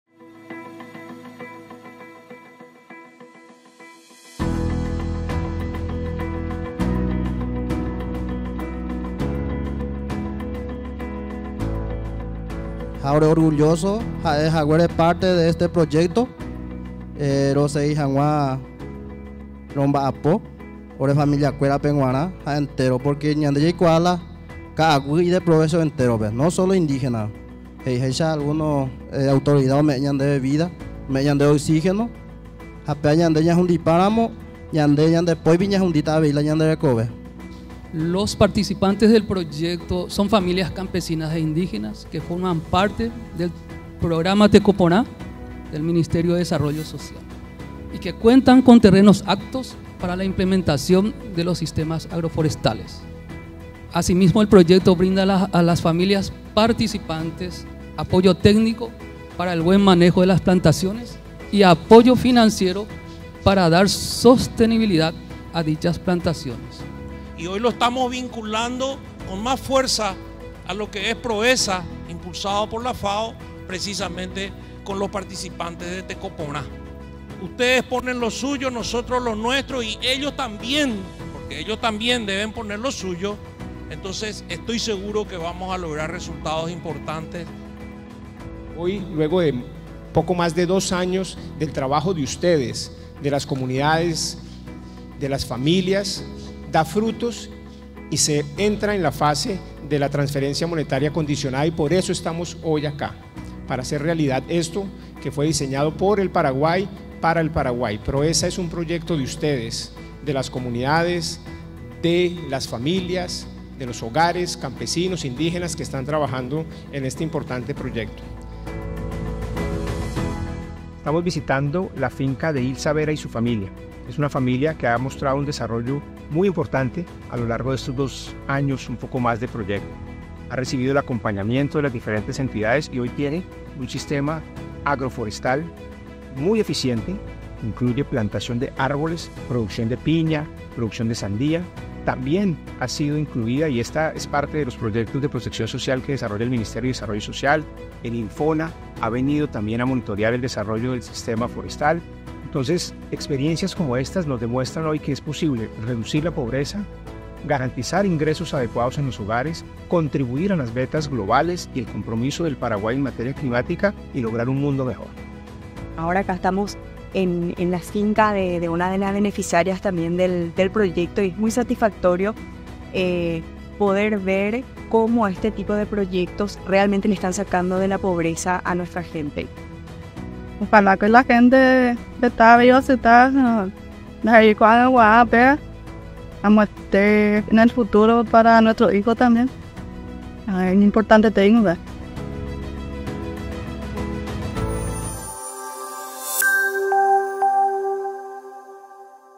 Proyecto PROEZA: transferencias monetarias ambientales a familias campesinas e indígenas
En el marco del Proyecto PROEZA Paraguay, implementado por el Gobierno con la cooperación técnica de la Organización de las Naciones Unidas para la Alimentación y la Agricultura (FAO), se realizó la entrega de Transferencias Monetarias Condicionadas Ambientales (TMCA) a familias campesinas e indígenas del distrito de Capiíbary, San Pedro.

El acto protocolar de entrega se llevó a cabo el miércoles 3 de julio, con la presencia de representantes de la Gobernanza del proyecto, autoridades nacionales y familias participantes. Tras el acto, se realizaron visitas a fincas campesinas e indígenas para verificar el avance exitoso de las plantaciones.
Las TMCA son una herramienta fundamental para ayudar a las comunidades más vulnerables a adaptarse a los impactos del cambio climático, proporcionando recursos financieros a las familias participantes.